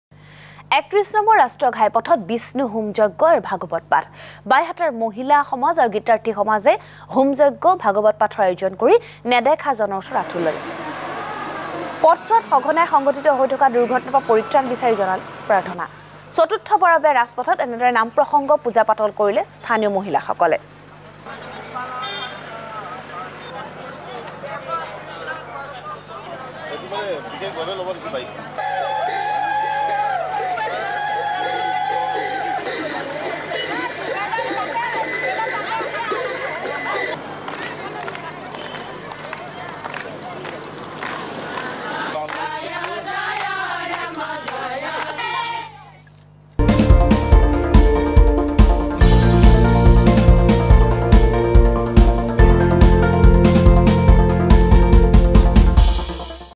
৩১নং ৰাষ্ট্ৰীয় ঘাইপথত বিষ্ণু হোম যজ্ঞ আৰু ভাগৱত পাঠ।
বাইহাটাৰ মহিলা সমাজ আৰু গীতাৰ্থী সমাজে ৩১নং ৰাষ্ট্ৰীয় ঘাইপথত বিষ্ণু হোম যজ্ঞ আৰু ভাগৱত পাঠ আয়োজন কৰি নেদেখাজনৰ ওচৰত আঁঠু ল’লে। পথচোৱাত সঘনাই সংঘটিত হৈ থকা দুৰ্ঘটনাৰ পৰা পৰিত্ৰাণ বিচাৰি জনালে প্ৰাৰ্থনা।